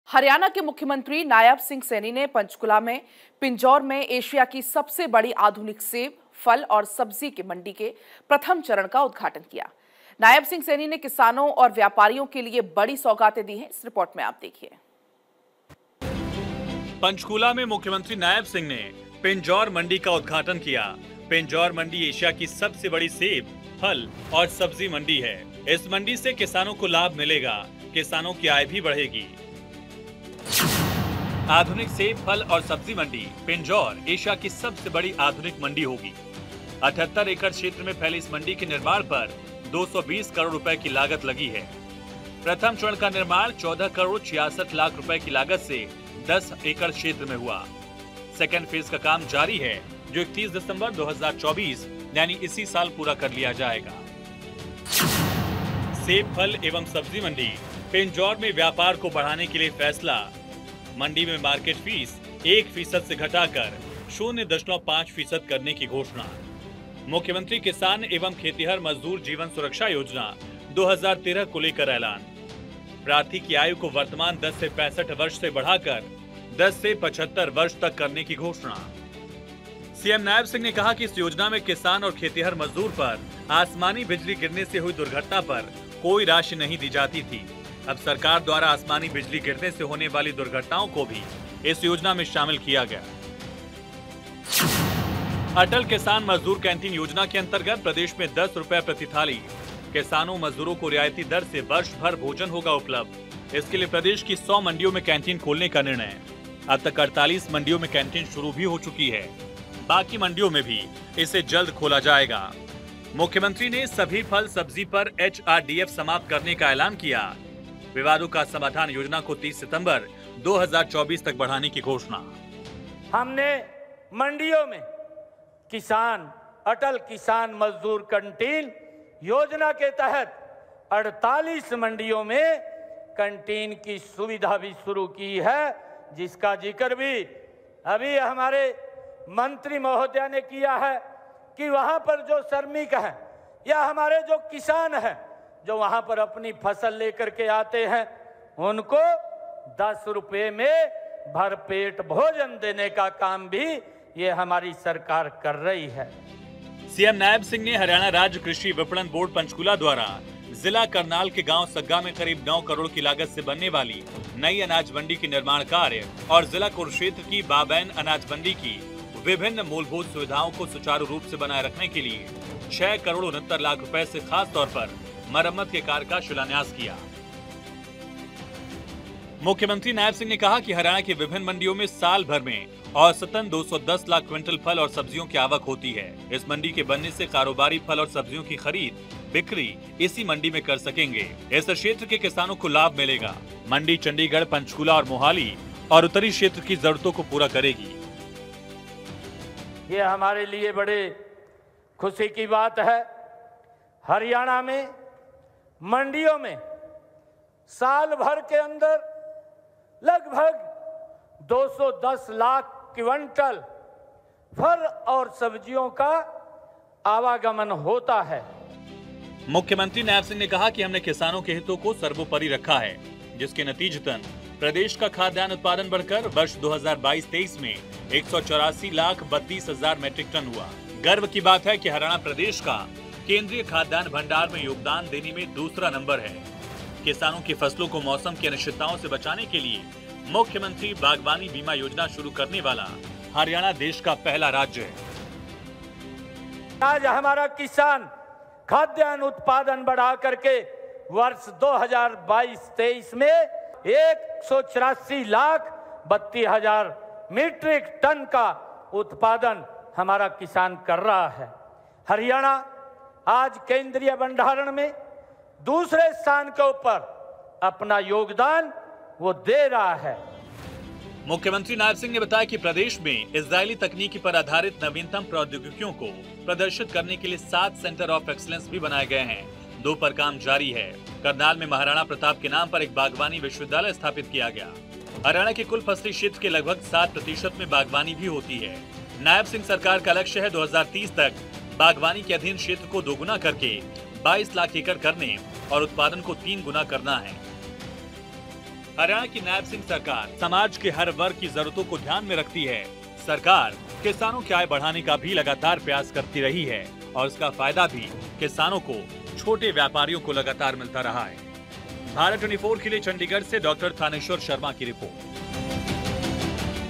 फल-सब्जी उत्पादक किसानों को बड़ी सौगात | Haryana | Chief Minister | Nayab Singh Saini | Farmers
हरियाणा के मुख्यमंत्री नायब सिंह सैनी ने  पंचकूला के पिंजौर में एशिया की सबसे बड़ी आधुनिक सेब, फल और सब्जी मंडी के प्रथम चरण का उद्घाटन किया...सीएम नायब सिंह ने किसानों और व्यापारियों के के लिए कई बड़ी सौगातें दी है,,,रिपोर्ट देखिए

Haryana Chief Minister Naib Singh Saini inaugurated the first phase of Asia's largest modern apple, fruit and vegetable market in Pinjore, Panchkula... CM Naib Singh has given many big gifts to farmers and traders, see the report

Anchor Name : Shikha Thakur  | Show Name : News | Time : 06: 50 AM | Date : 20/07/2024 | Published By:Yogesh Mishra